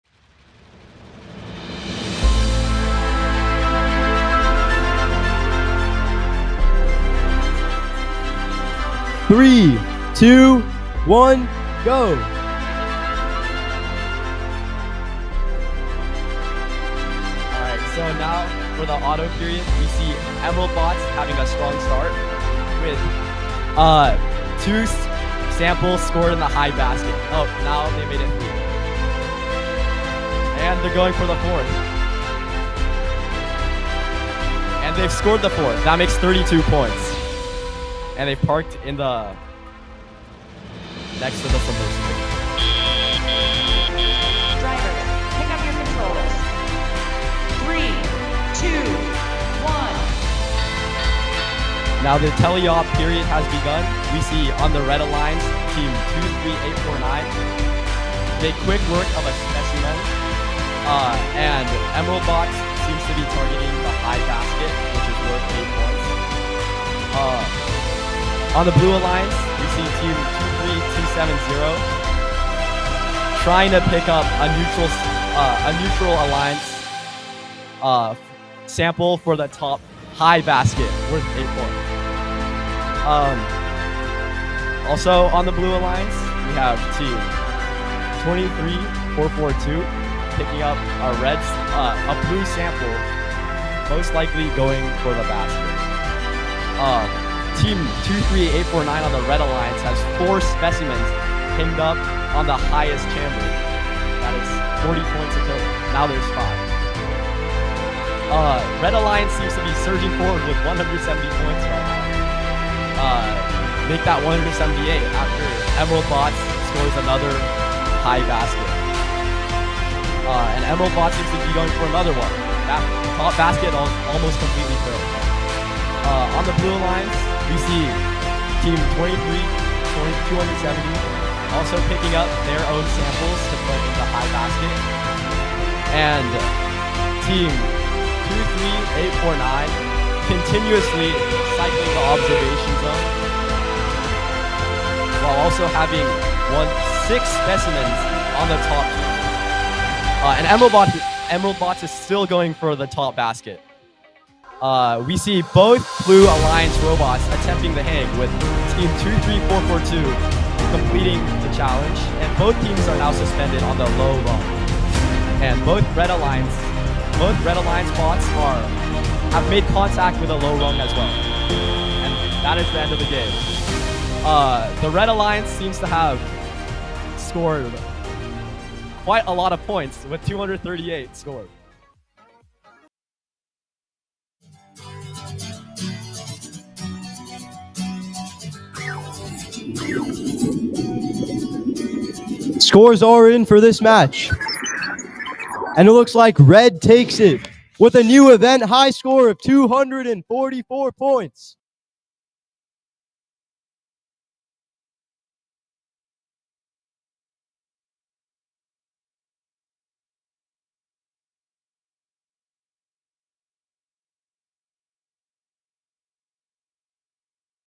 FTC New Washington High Score | 23849 Droid Force + 19708 Emerald Bots
5th highest score in the world